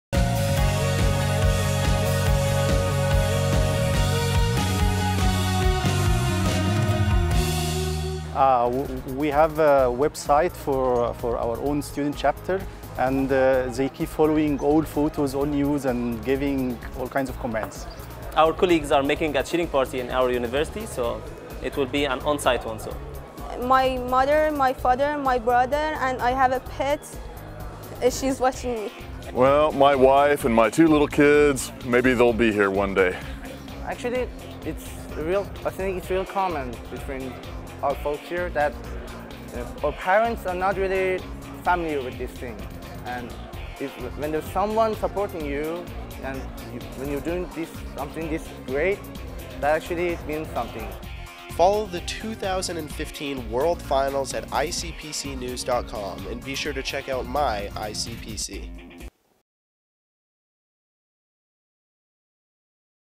2015 ICPC: Who Is Watching at Home
2015 World Finalists have lots of support from their family and friends back home, including some very loyal pets! Contestants and coaches tell us who is on the home team.

Faculty of Computer and Information Science: Ain Shams University :10

Arab Academy of Science and Technology, Alexandria :19

University of Tehran :23

Rice University :29

Shahid Beheshi University :36